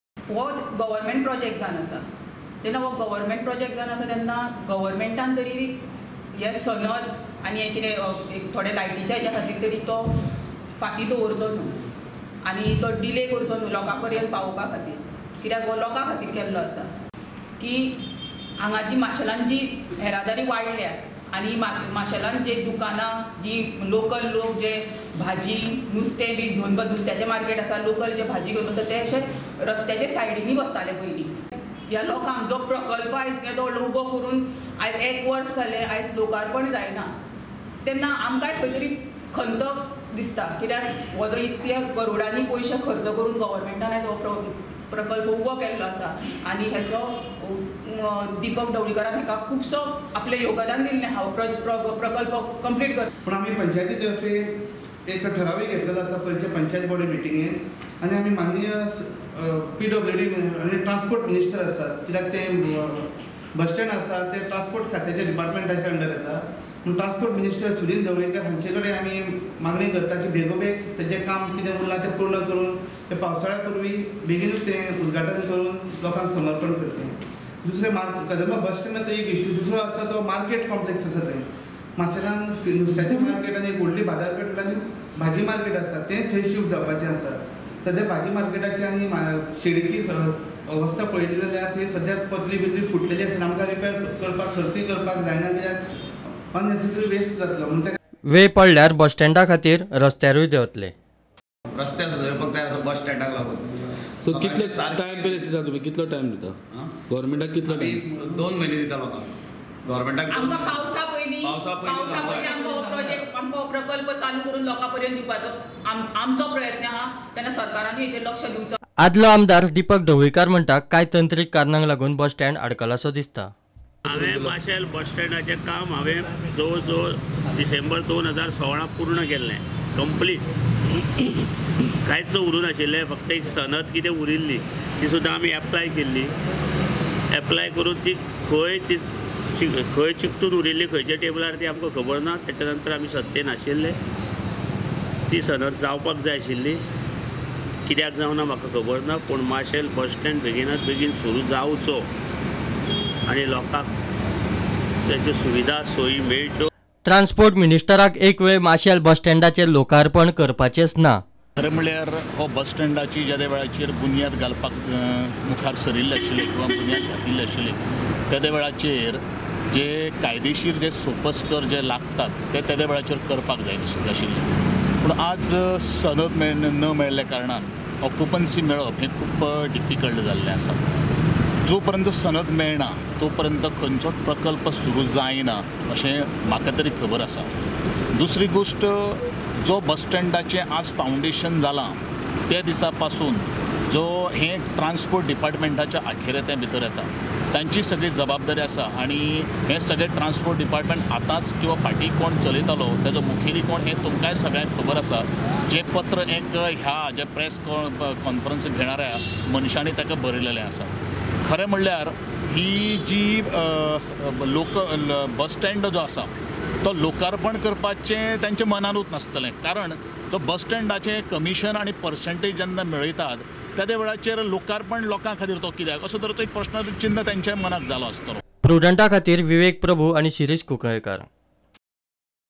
UNENDING ‘WAIT’ OF MARCEL BUS STAND
Marcel bus stand has been completed and awaiting use since long. Project was specially designed to accommodate vegetable market and flower vendors. However, despite being ready the structure cannot be used due to some technical reasons. Local representatives have urged government to complete and bring the project in service.